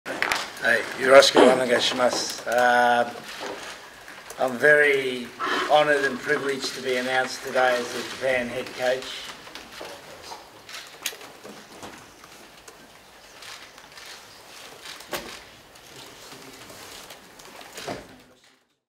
「より速いラグビーを」 ジョーンズ次期ＨＣが就任会見 ラグビー日本代表
ラグビー日本代表の次期ヘッドコーチ（ＨＣ）就任が決まったエディー・ジョーンズ氏（６３）が１４日、東京都内で記者会見に臨み、２０２７年ワールドカップ（Ｗ杯）オーストラリア大会に向けて「まずは８強を目指す」と抱負を述べた。記事
【時事通信映像センター】